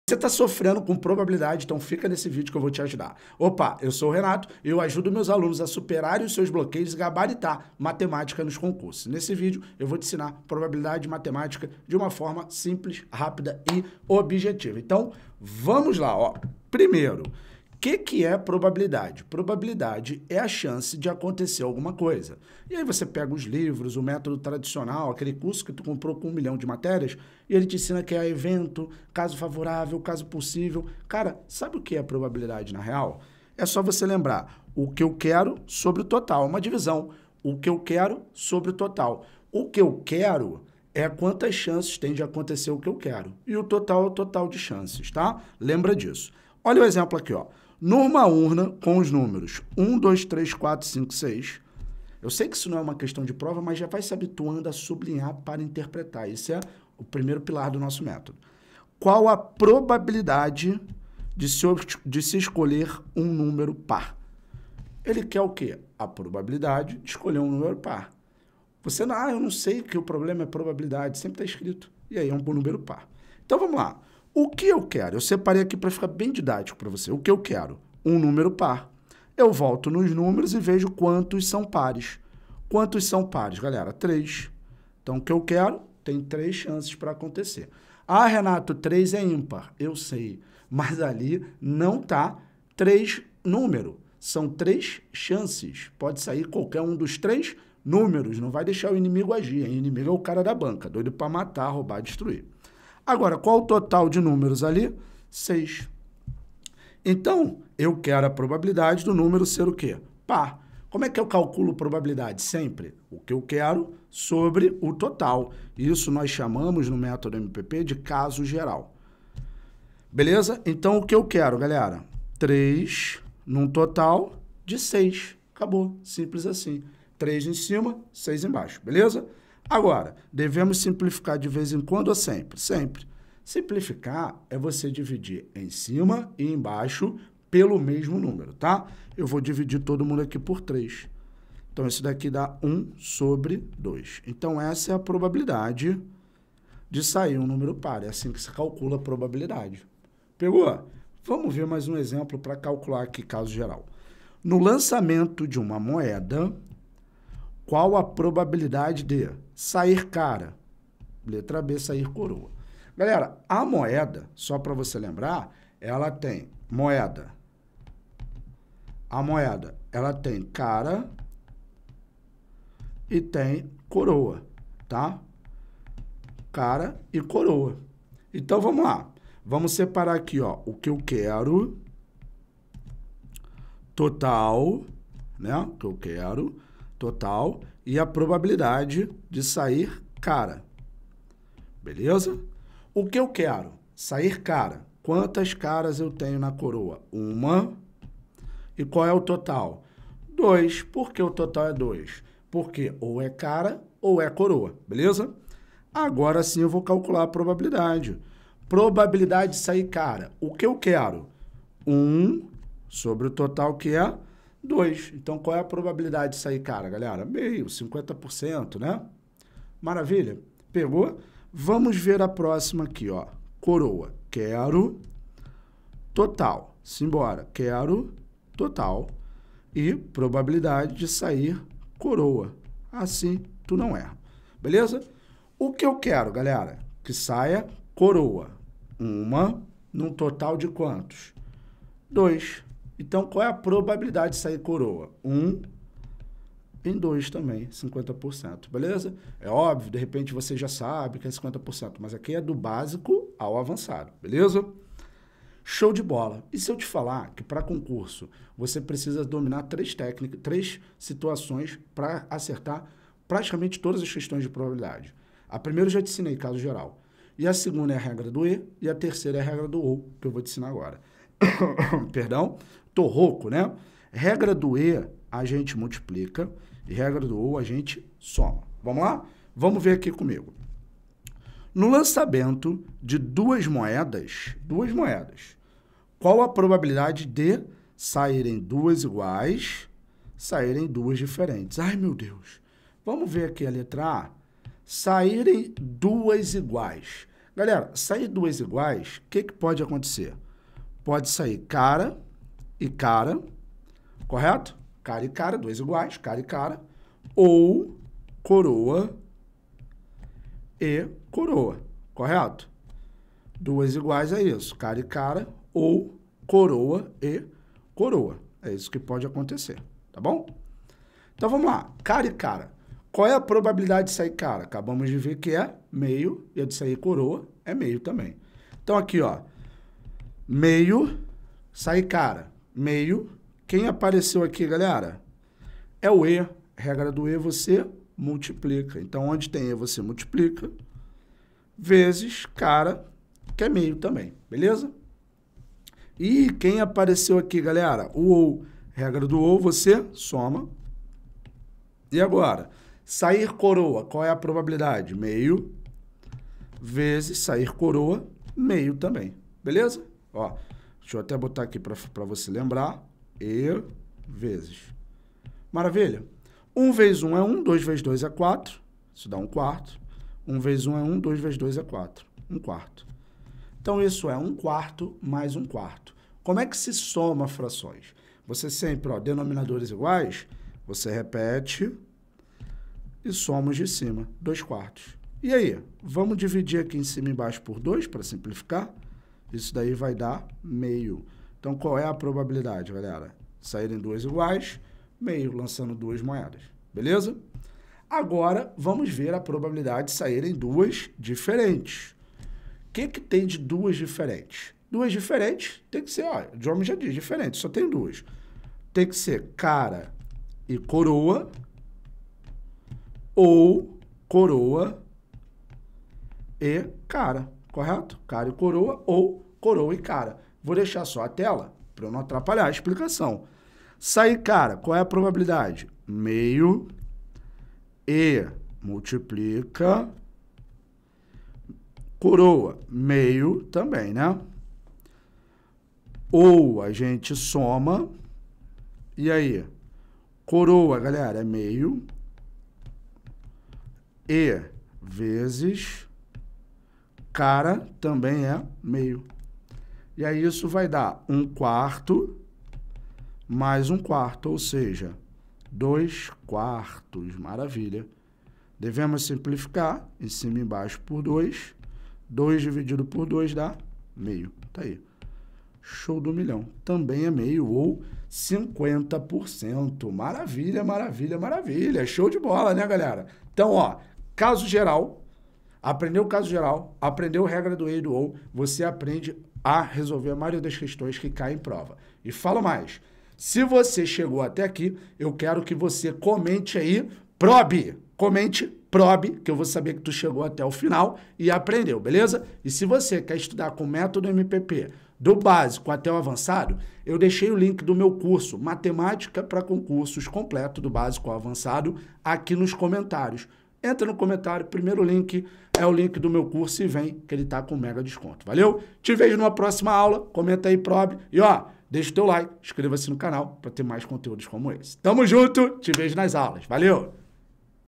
Probabilidade Matemática.
Você quer Aprender Probabilidade?

Sabia que tem como Aprender Probabilidade em 08 minutos?

Então você precisa assistir esse vídeo onde nosso professor, Renato Oliveira, mostra todos os segredos para aprender Probabilidade e acertar as questões no seu concurso.

Você terá um passo a passo para aprender essa matéria e nunca mais errar nenhuma questão.

Agora basta dar o play no vídeo e desvendar com o Método MPP os segredos da Probabilidade Matemática.

00:00 Sofrendo com Probabilidade?
00:10 Inscreva-se no Canal e Deixe o seu Like
00:20 O que é Probabilidade?
00:33 Caso Geral
00:48 Numa urna com os números 1, 2, 3, 4, 5 e 6, qual a probabilidade de se escolher um número par?
02:33 No lançamento de uma moeda qual a probabilidade de:
04:31 Regra de "e " e "ou"
04:42 No lançamento de uma moeda qual a probabilidade de:
07:55 Revisando
08:11 Curso com Desconto Link nos Comentários